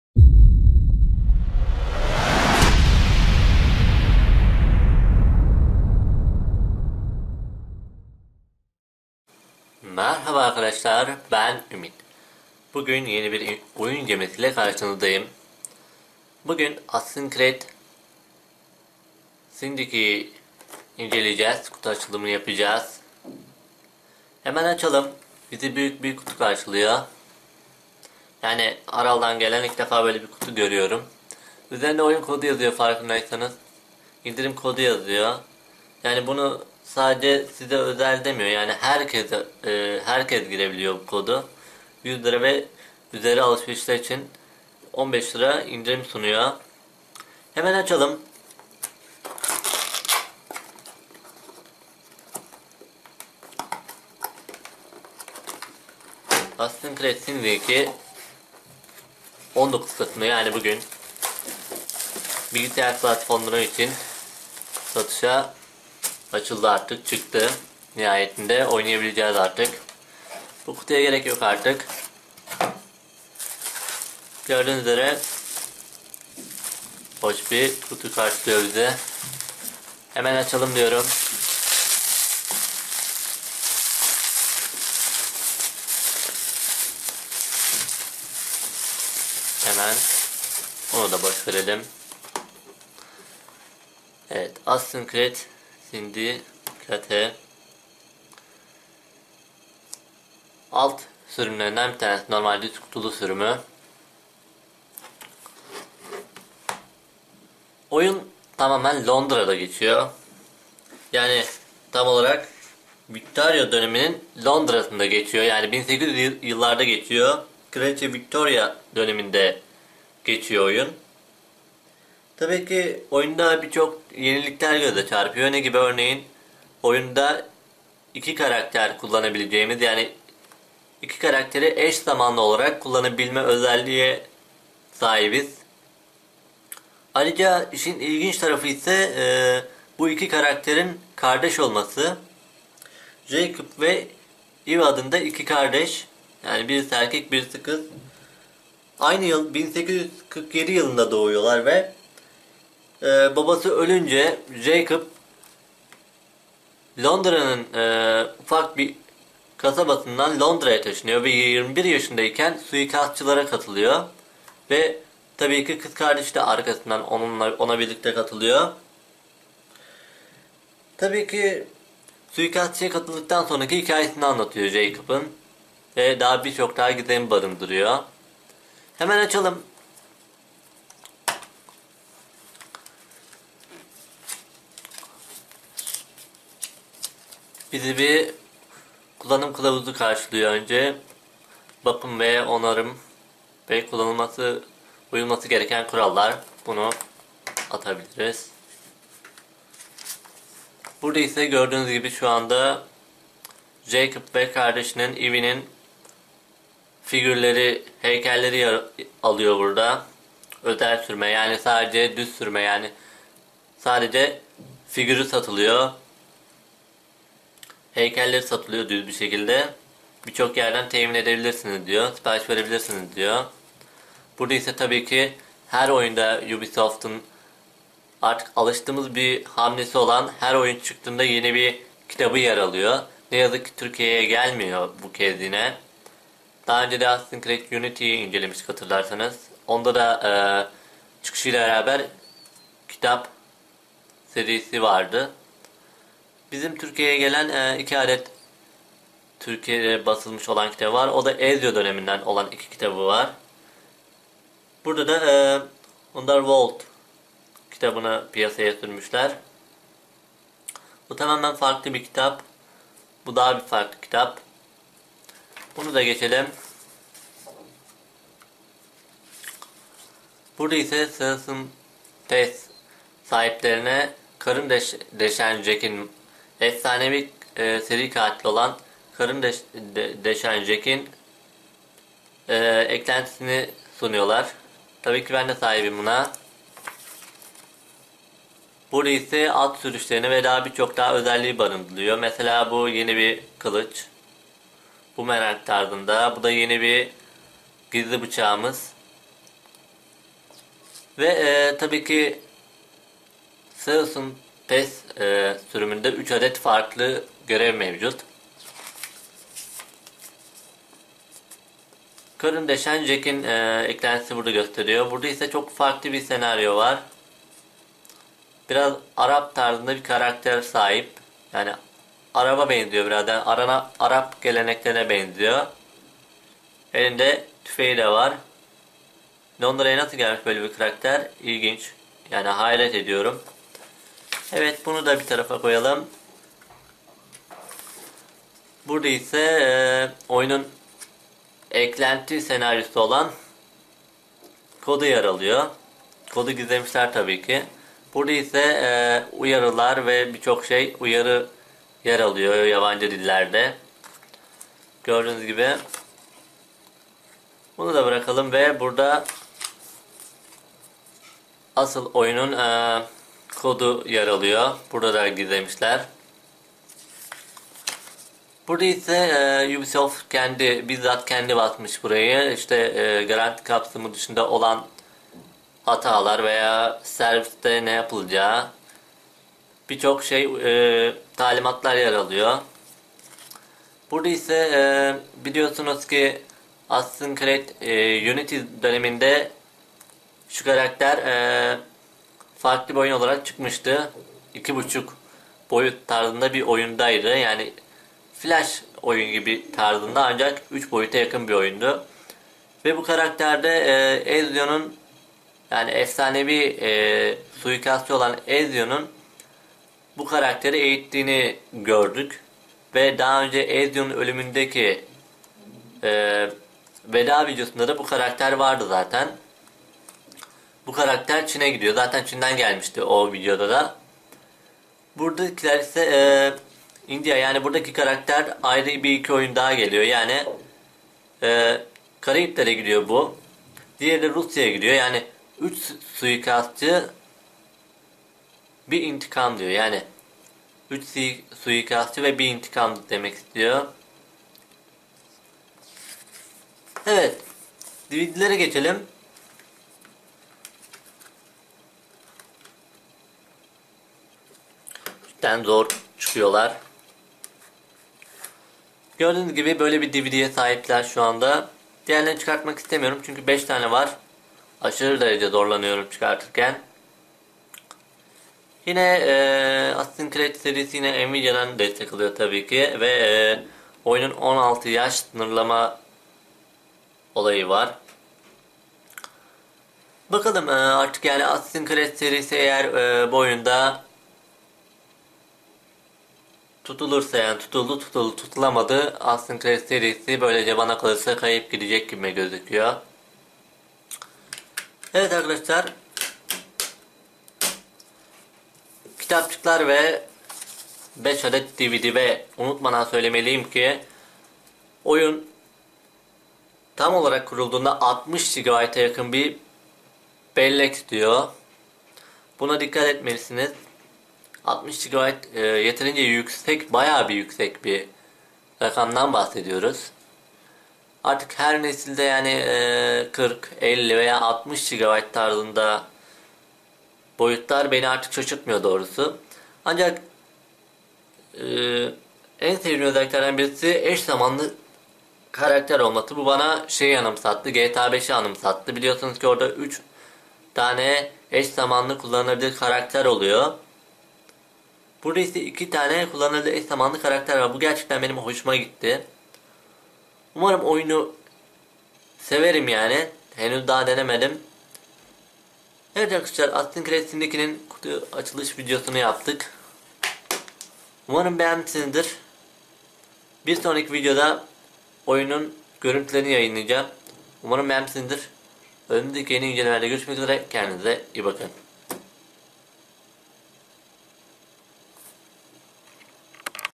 Assassin's Creed Syndicate PC Special Edition Unboxing
Assassin's Creed Syndicate 19 Kasım (Bu gün) itibariyle PC platformlarına çıktı nihayetinde ağız tadıyla oynayabileceğiz.:) Bu gün Assassin's Creed Syndicate Special Edition İncelemesi yaptık.- İyi seyirler.